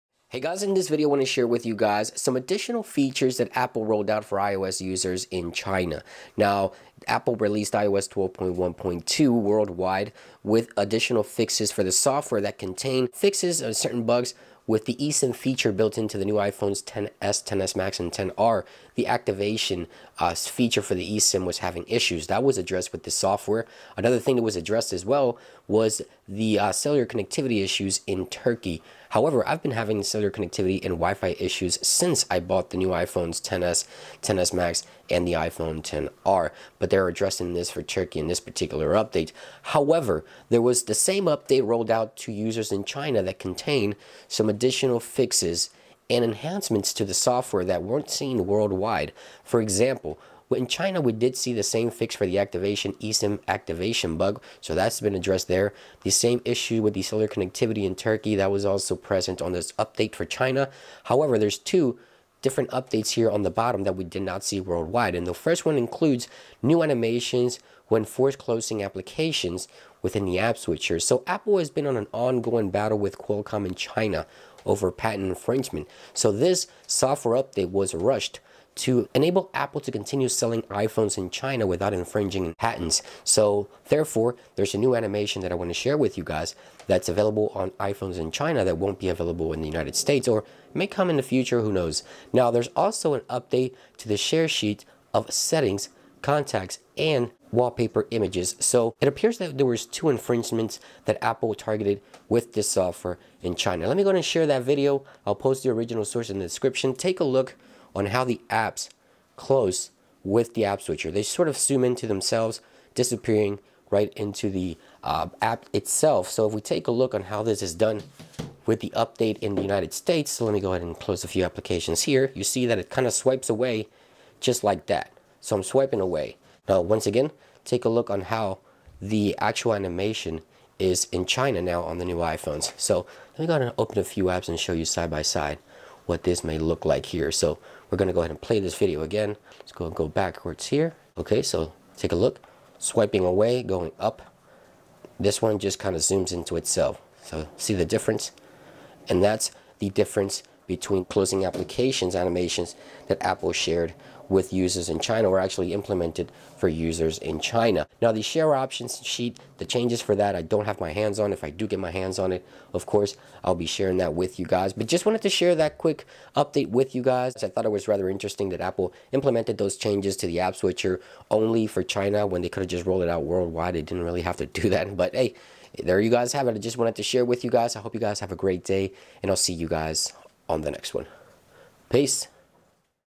iOS 12.1.2 Exclusive NEW App Switcher Animation in China
In this video I share with you New updates pertaining to iOS 12.1.2 in particular new apps switcher animations in China